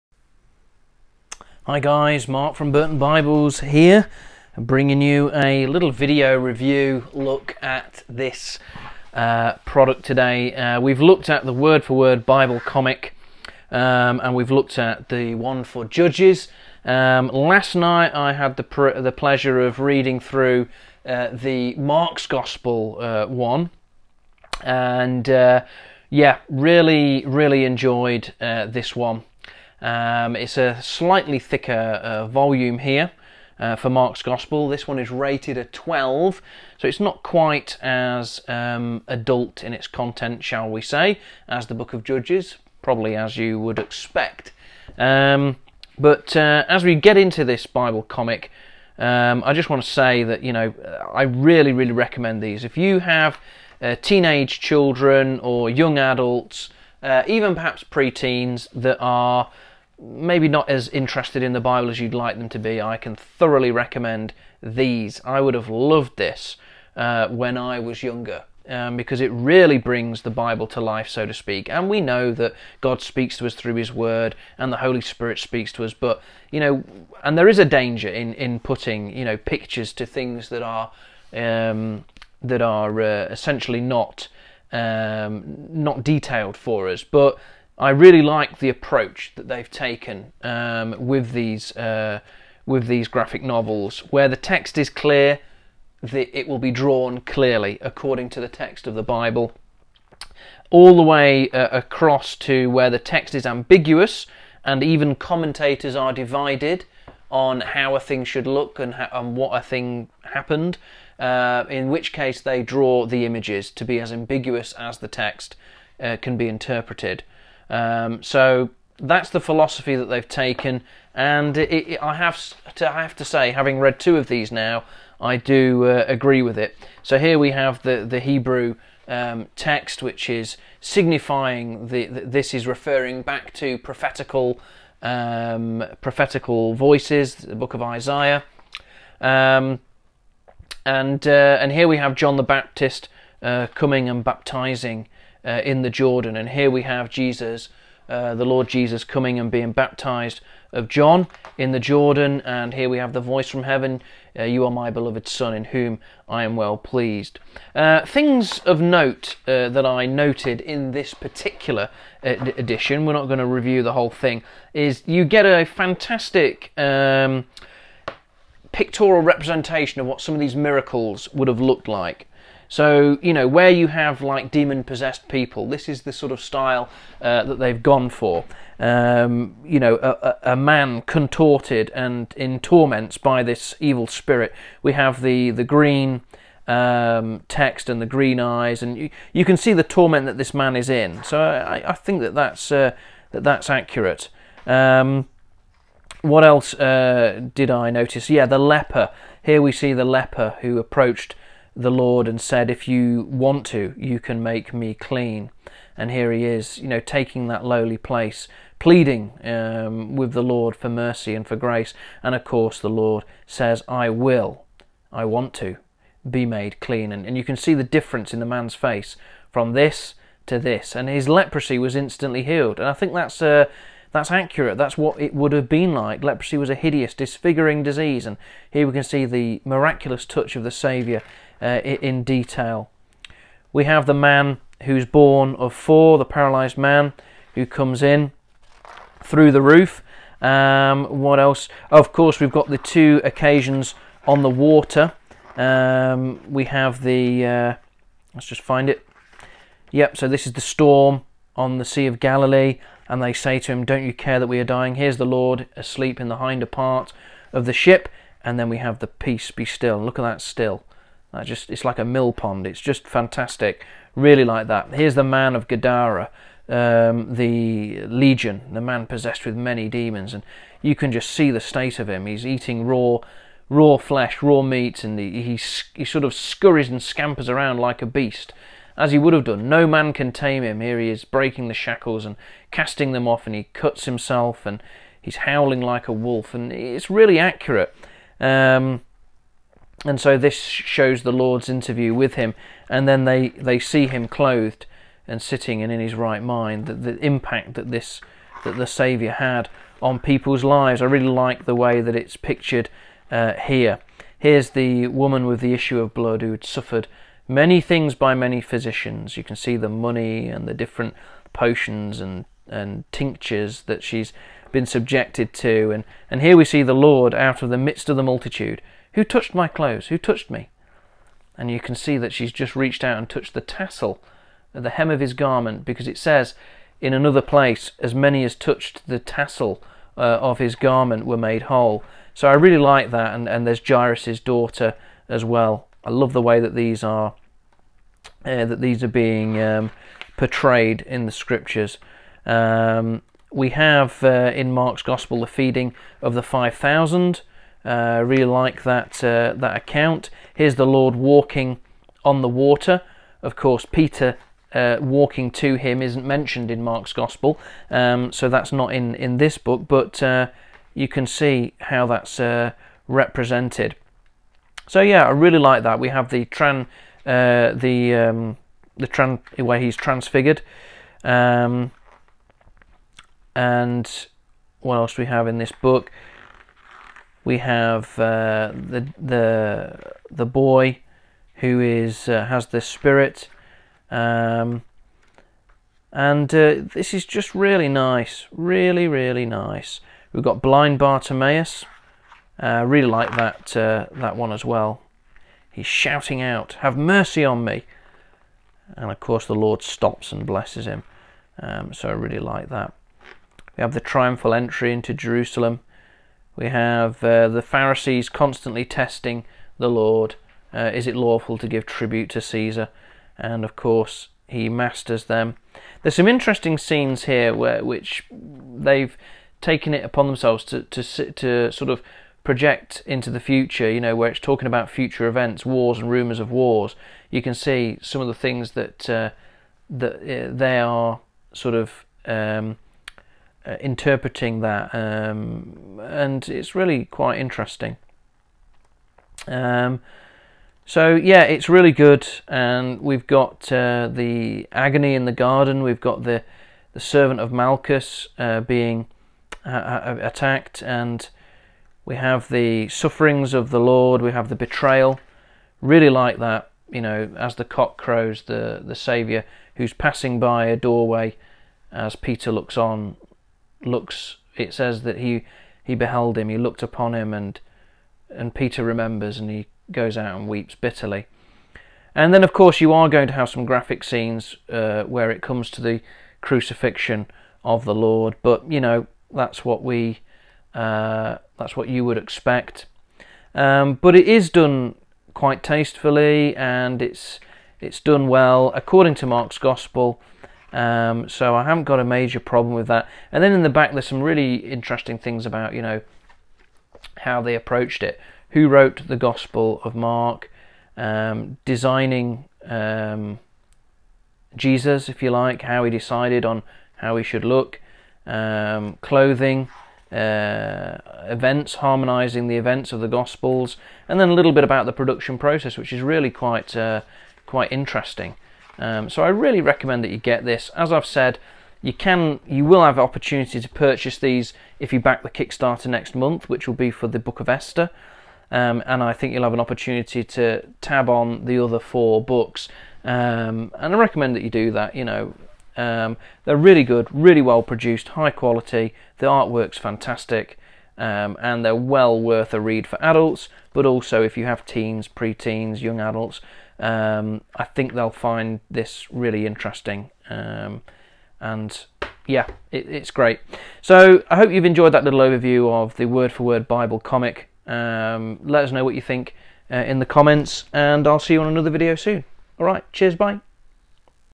Word for Word Bible Comic: Mark's Gospel
Continuing our look at these great comics, we look at the Gospel of Mark!

We are based in the Midlands, UK but can ship across Europe and even globally!